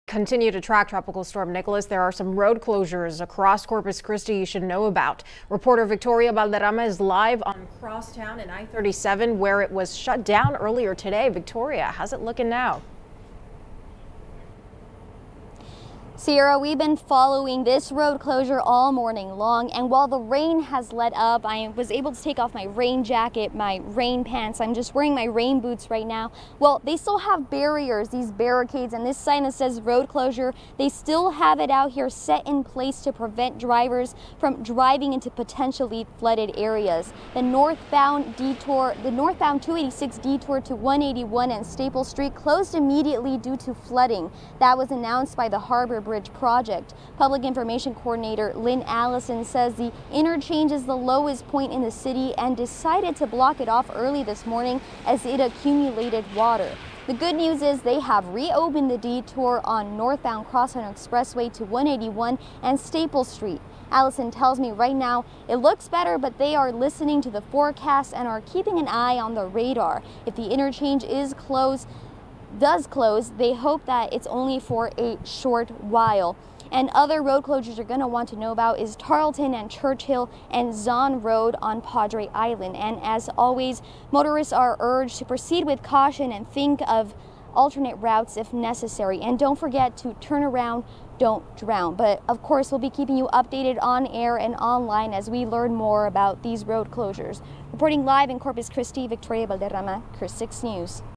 Road closures announced before Nicholas arrives in Corpus Christi
Several major road closures were announced Monday morning before Tropical Storm Nicholas arrived in Corpus Christi.